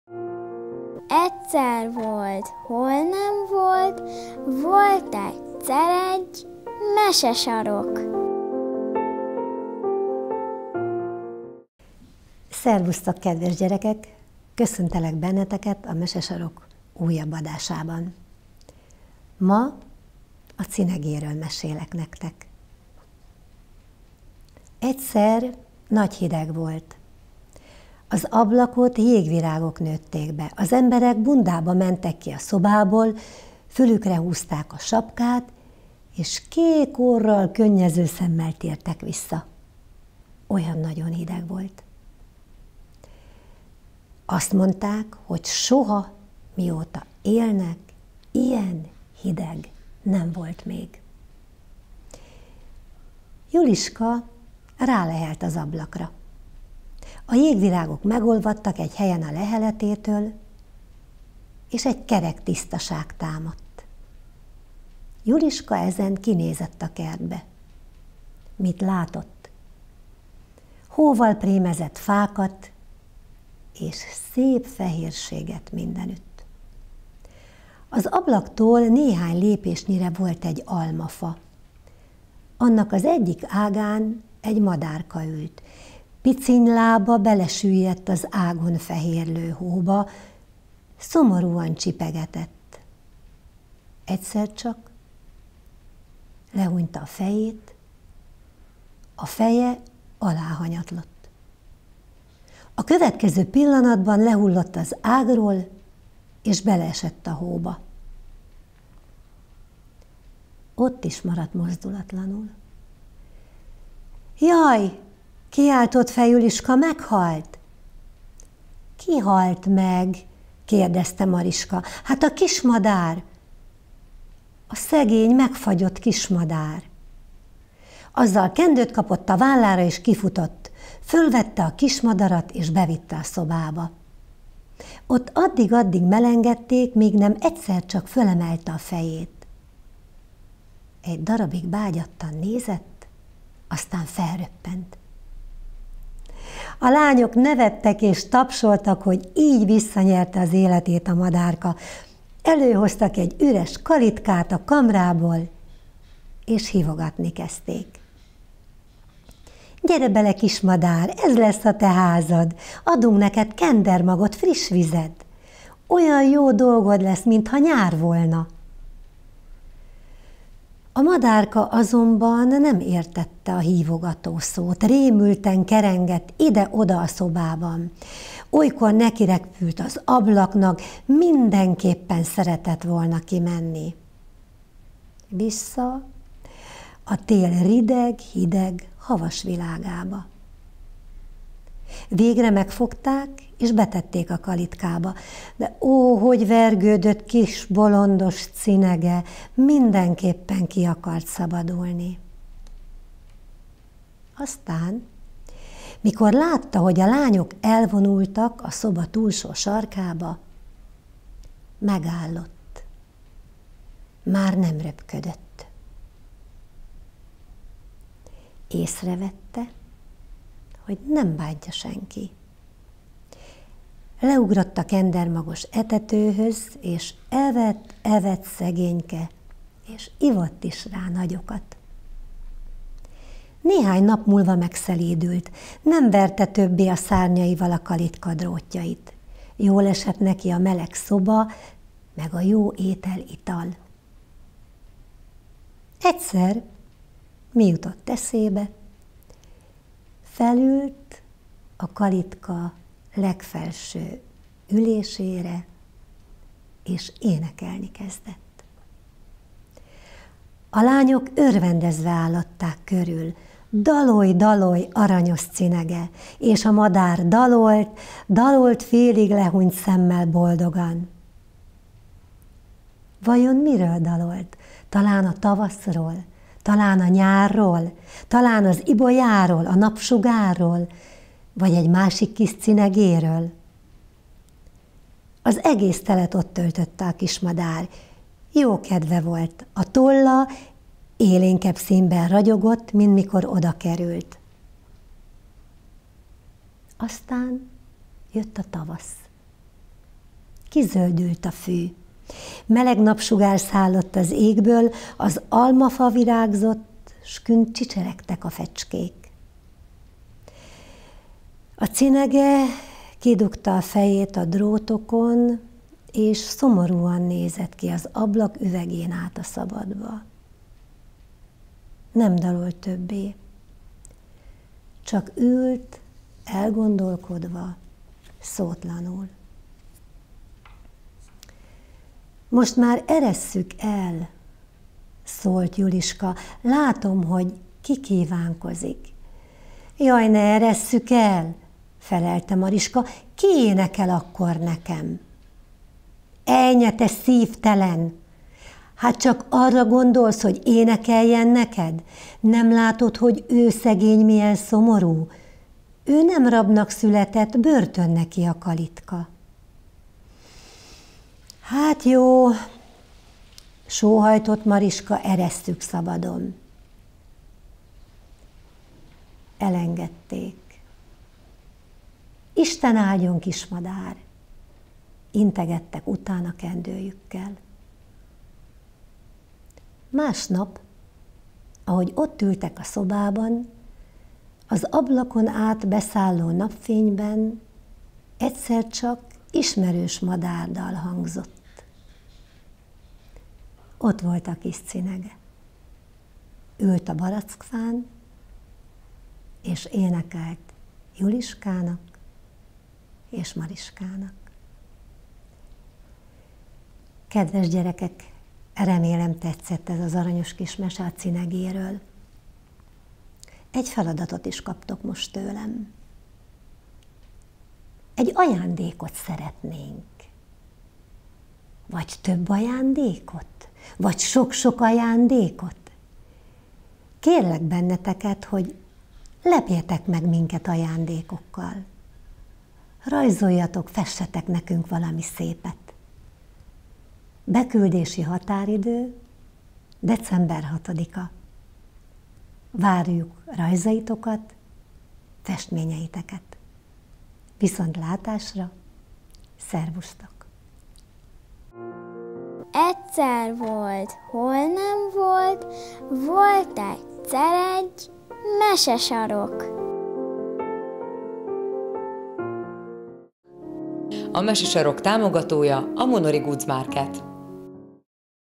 Mesesarok - 2017. november
Mesesarok rovatunkban Szentirmay Cecíliától egy újabb szép mesét hallhatnak a gyerekek. Ezúttal is van pályázati lehetőség.